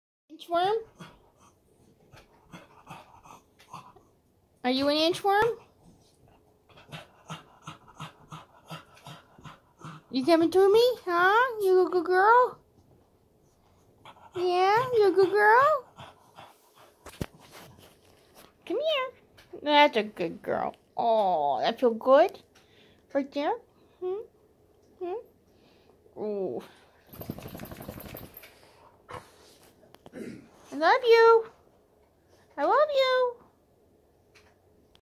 Cocker Begging for Food
this is how my dog begs us to feed her early